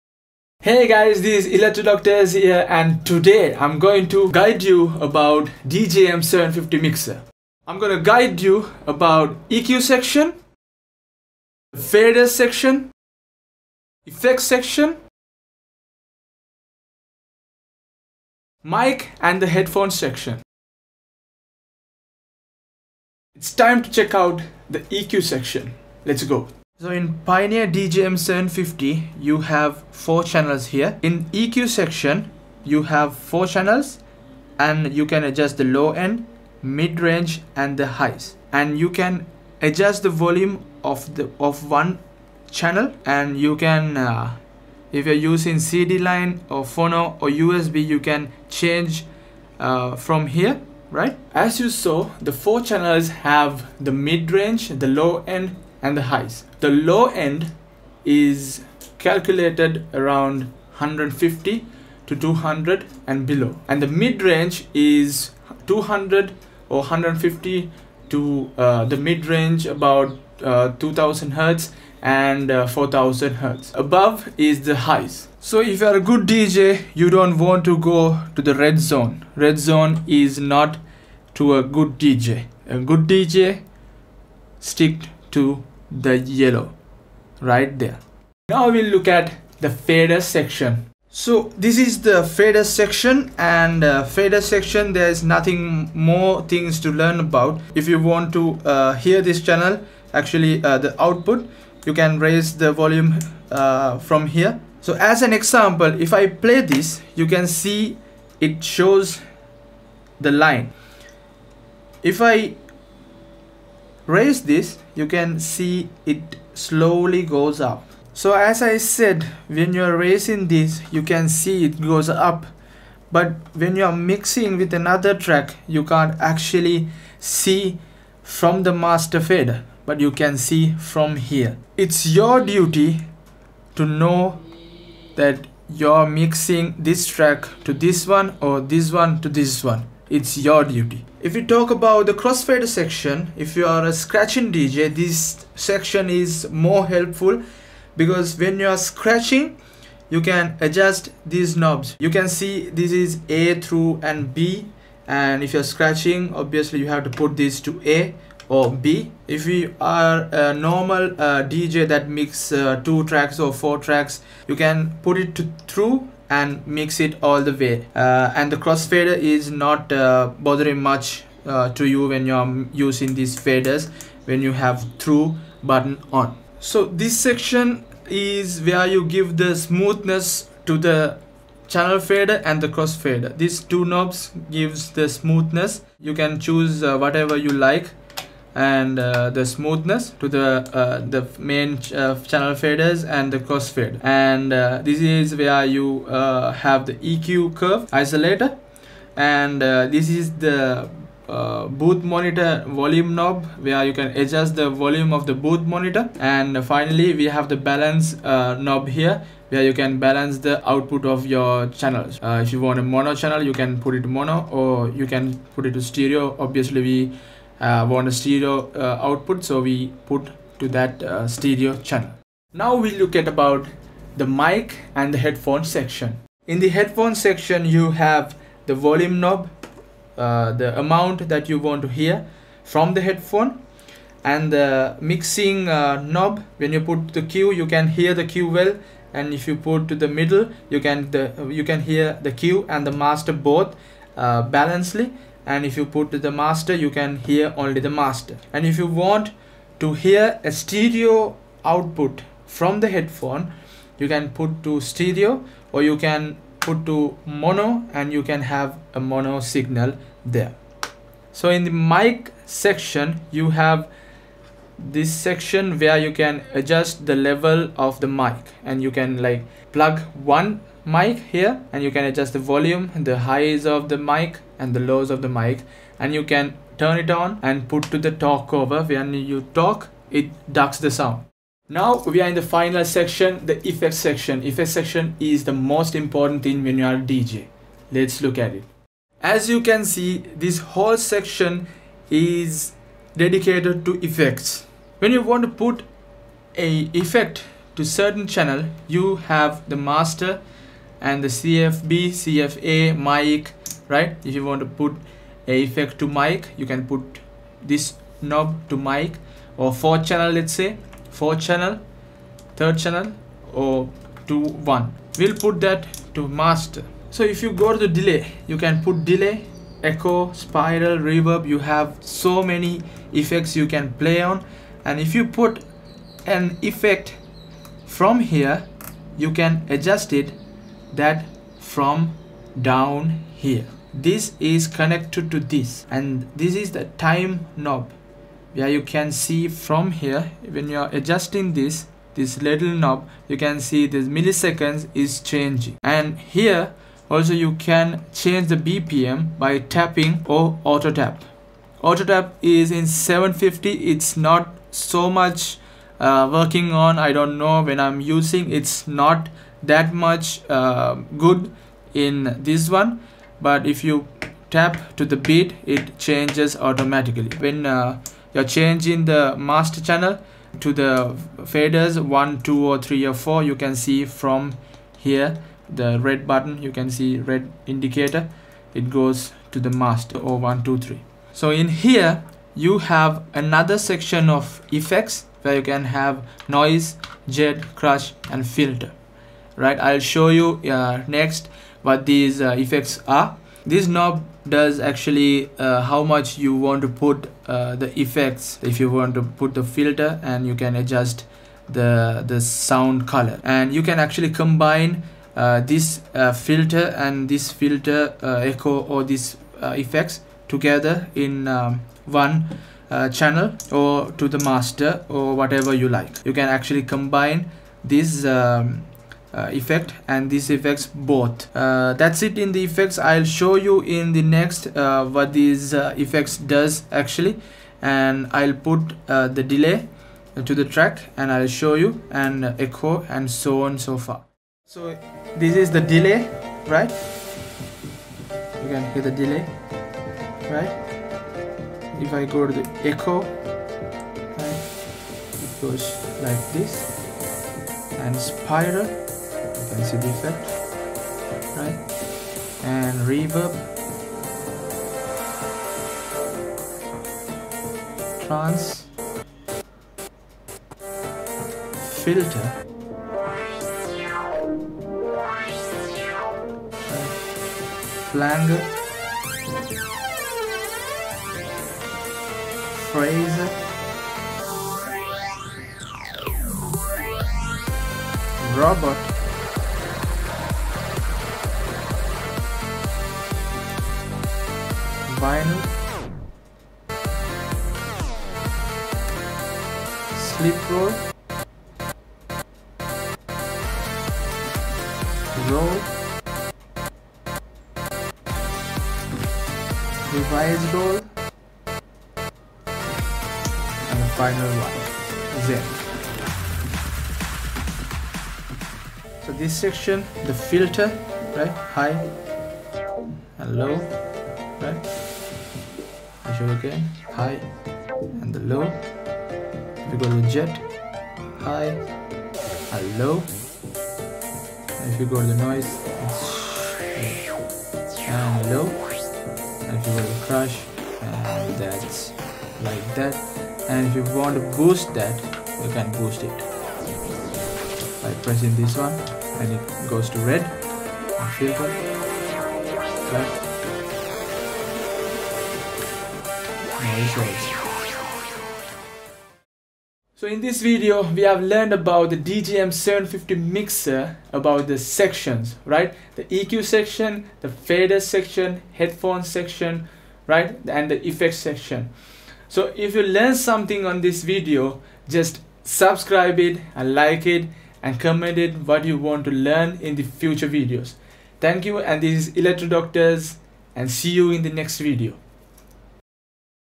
Lets Spin the Decks #02 - Mixer Sections (Pioneer - DJM750)
Lets check about the mixer (Pioneer DJM750), in this video I've guided through what's there in the mixer. Check out the sections of the mixer where it has the EQ Section, Faders Section, Mic & Headphone Section & the Effects Section.
This is Pioneer DJM750 mixer & you will have the same features in some latest Pioneer (DJM900) or other DJ Mixers. Latest pioneer mixers have some more features than the DJM750 Mixer but the same theories applies.
This time we thought to do this in English language since it's the universal language. DJ is an ART and i have learnt lot of things alone with my own experiences, there might be mistakes and don't hesitate to correct them.